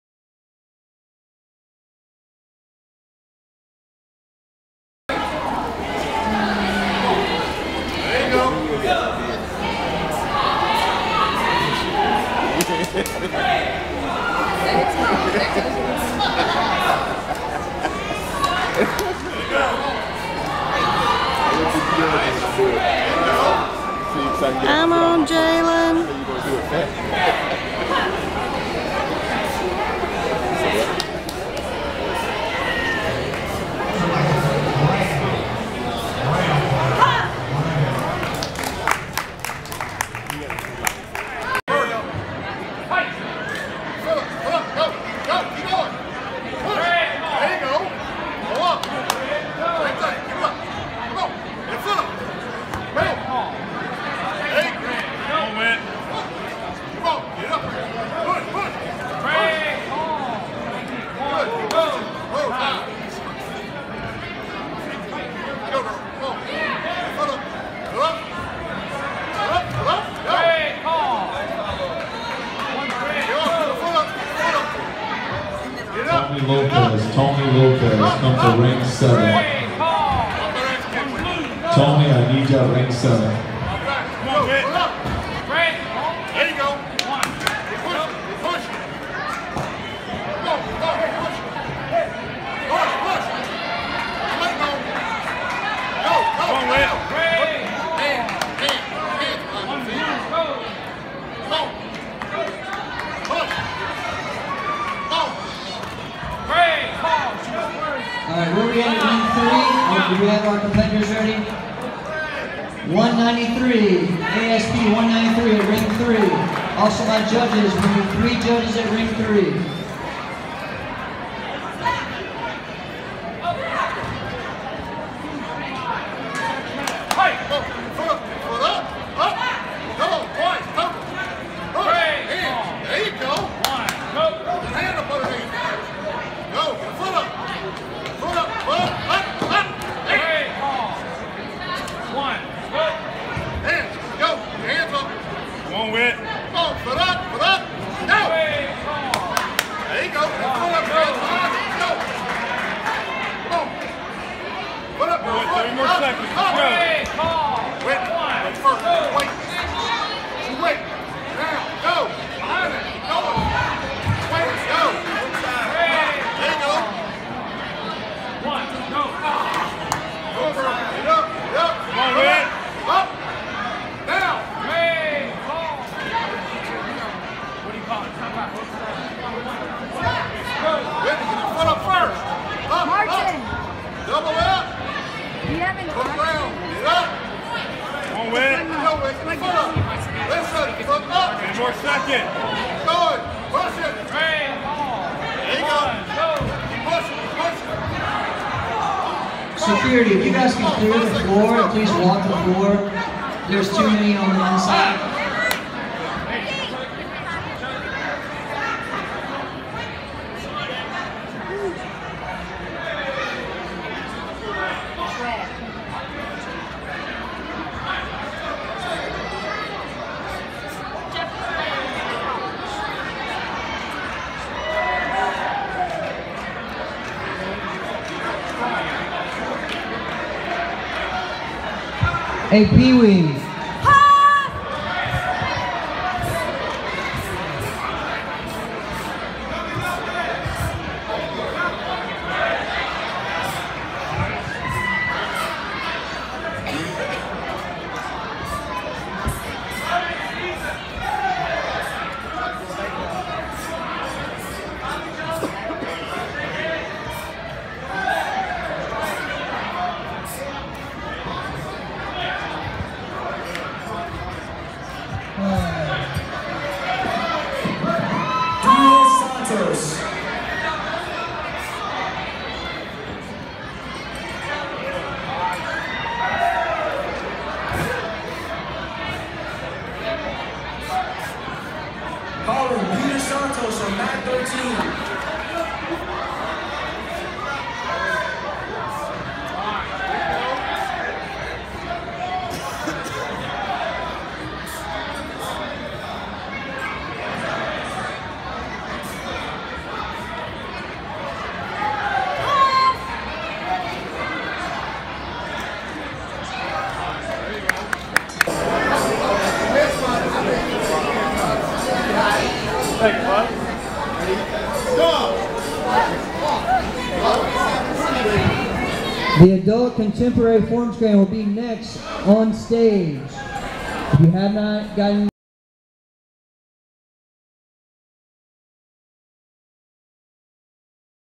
Slug Fest Karate Nationals 2016
Slug Fest Karate Nationals 2016 in New Orleans.
I competed in Korean forms, creative weapons and point sparring.